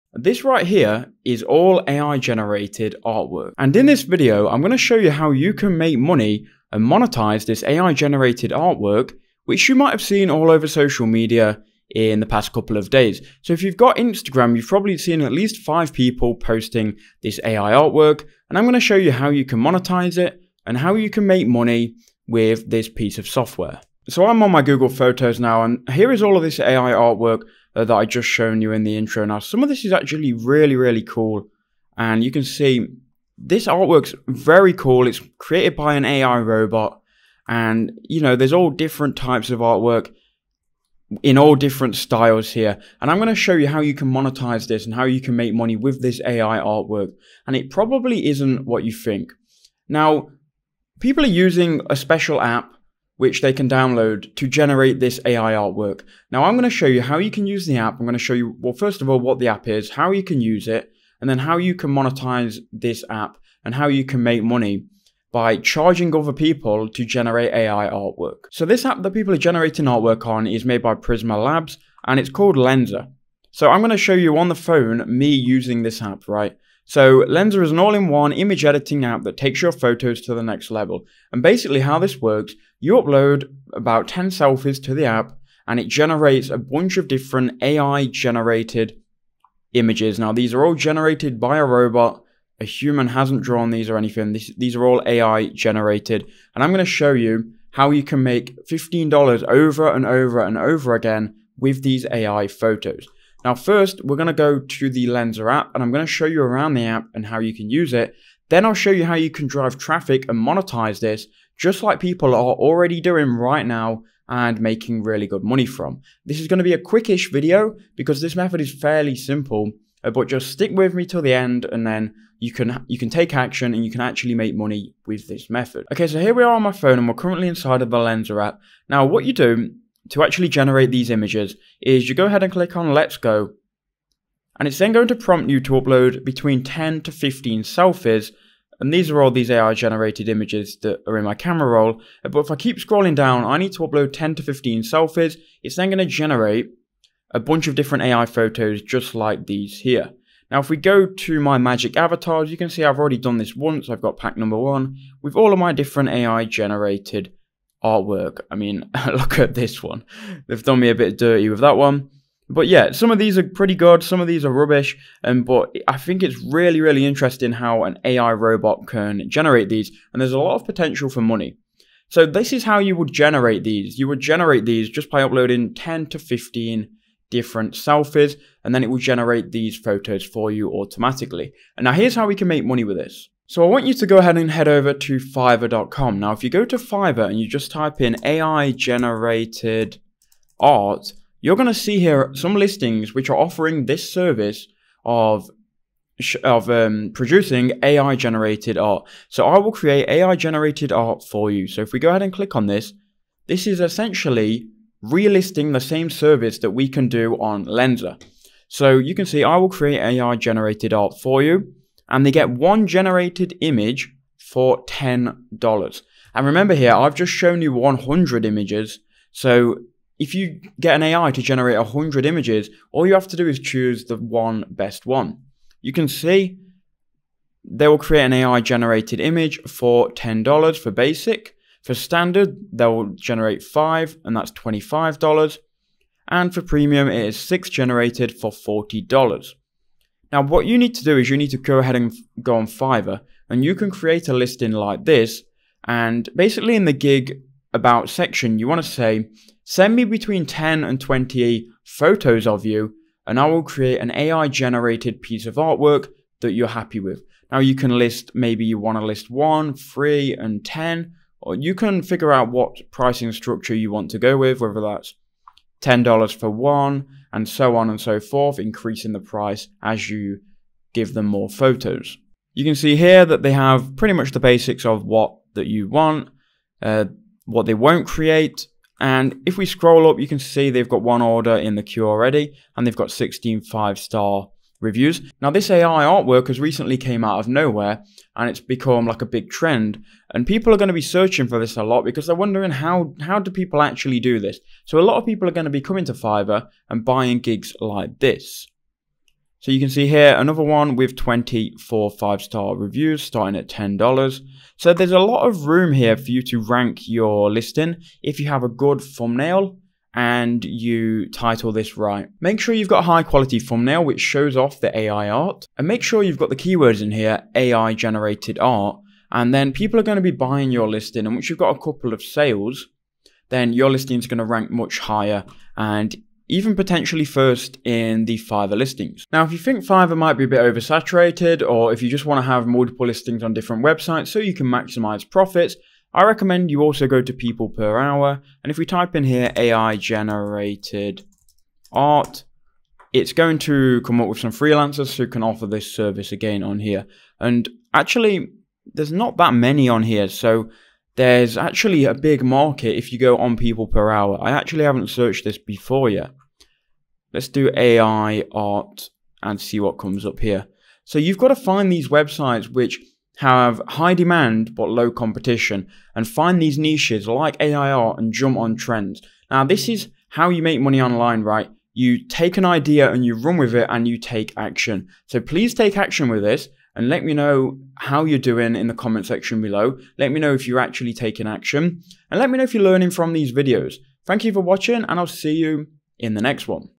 Get Paid $15.00 Every 10 Minutes Using AI-Generated ART (Make Money With Lensa App)
Learn how to earn $15.00 in 10 Minutes using AI-Generated art. This is a hot new trend right now and the potential is there to make money while a lot of people are talking about it. You have probably seen these AI drawings on your personal Instagram feed. Use this hype to profit and make some easy money online.